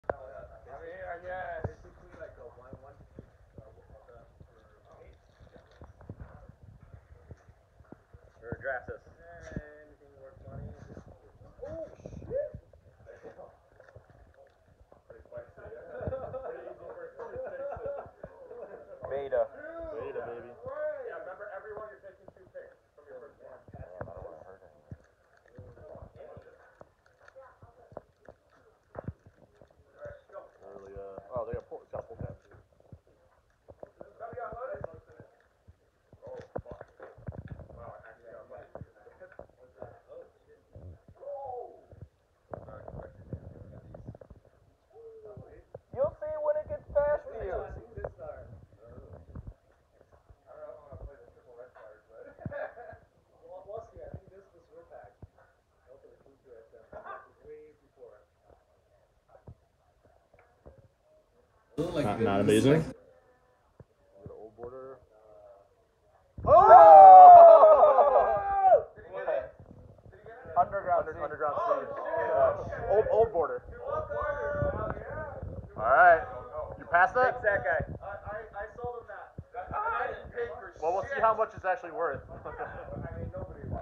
Mad Lad Frank Drafts A Magic 30 Pack In Chaos Draft And Hits A Reserve List MONEY CARD!
Ebay Store